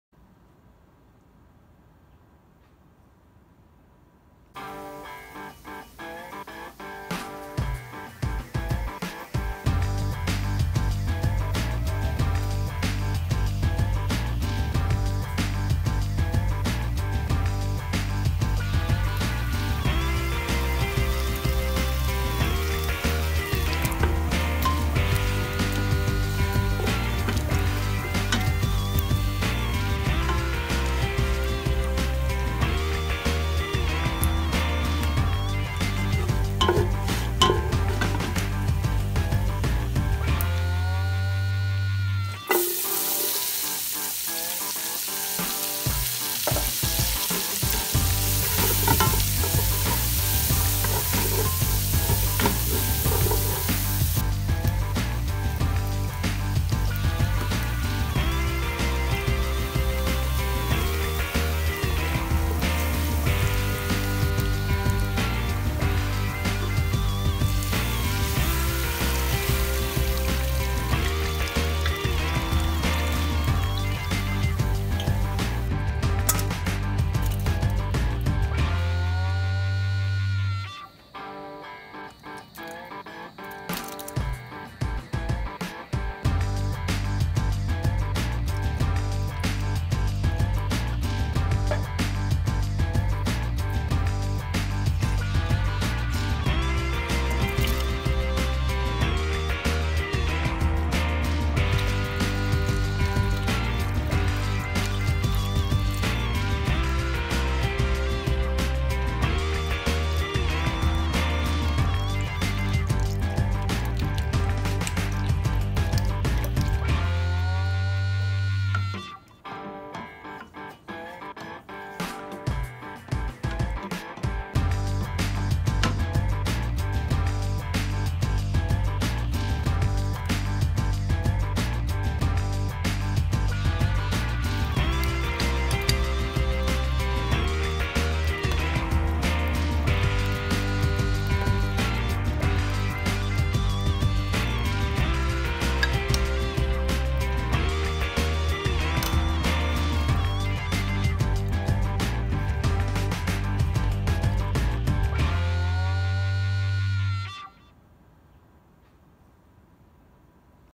DadCooks - Home Style Clam Chowder
Easily another family favorite! Clam chowder always hits the spot.